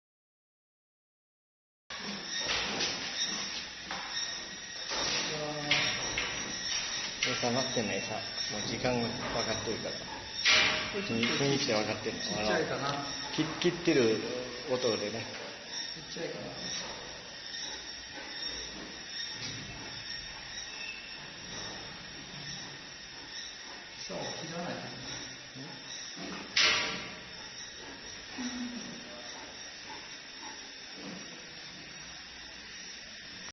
3D Panda@Yaan #18
食事をおねだりする姿もかわいい。